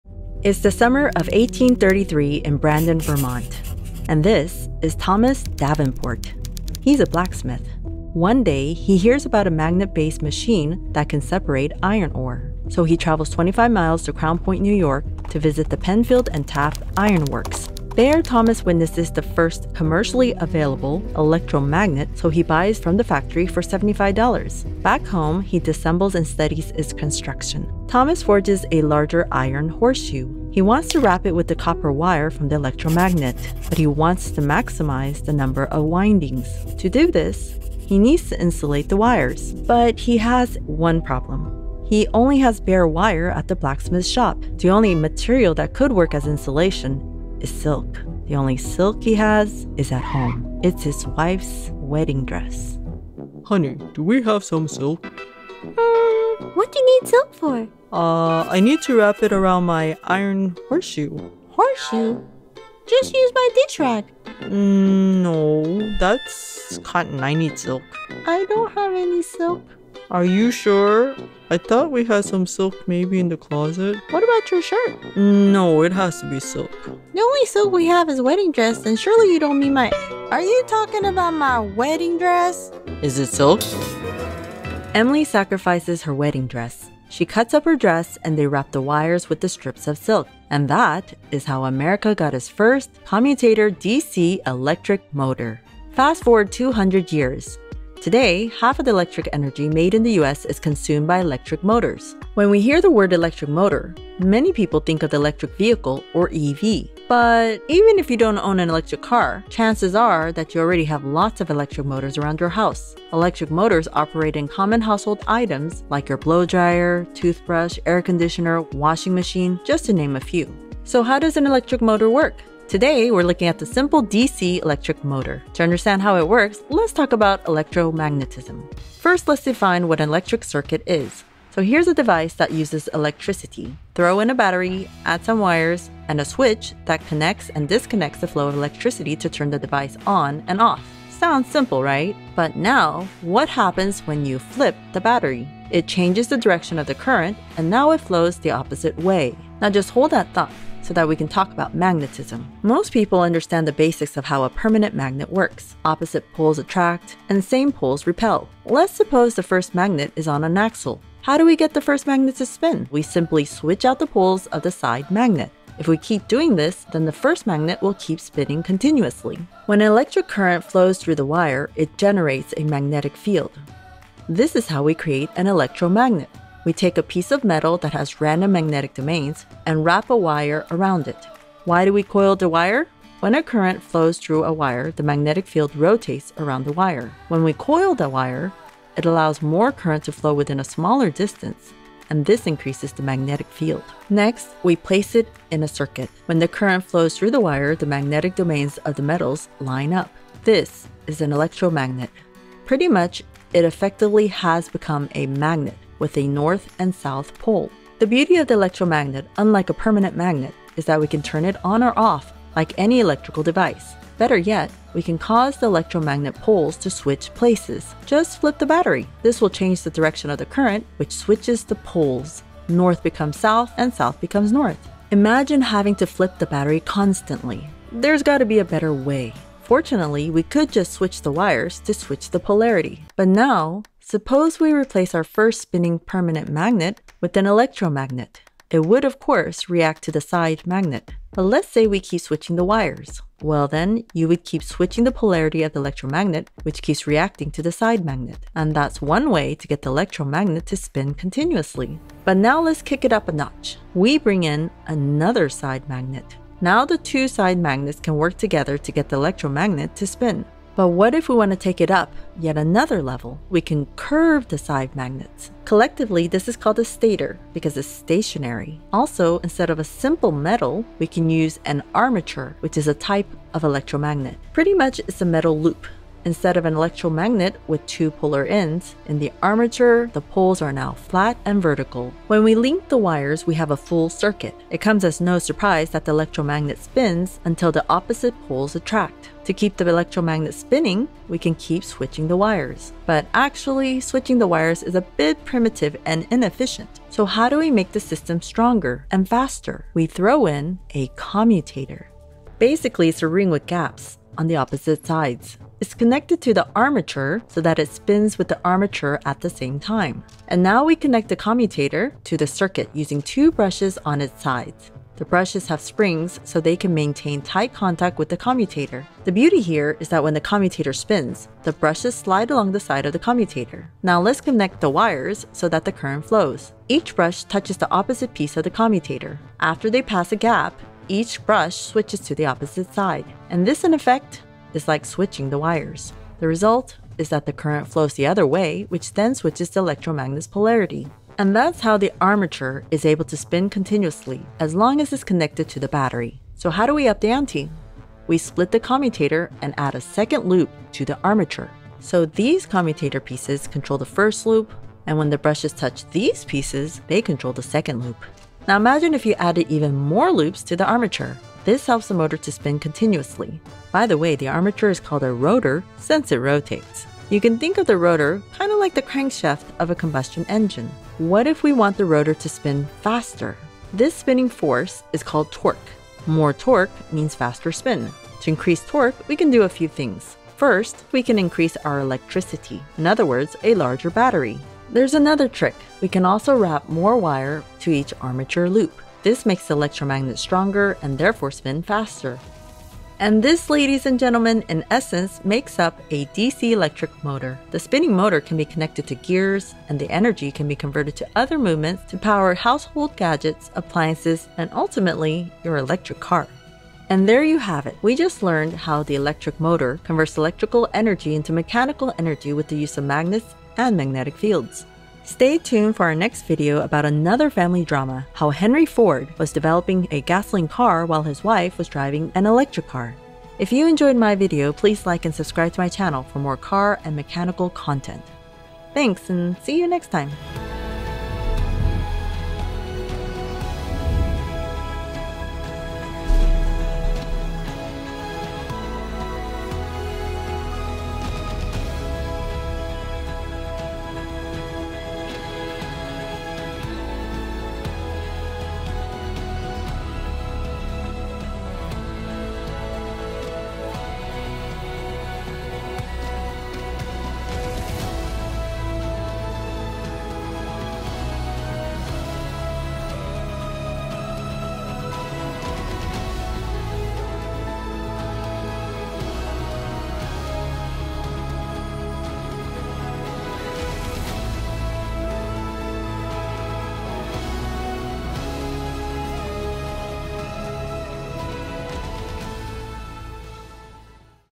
Electric Motor Made from Wedding Dress
See how a simple DC motor works.  Also learn how a wedding dress contributed to the invention of America's first DC electric motor.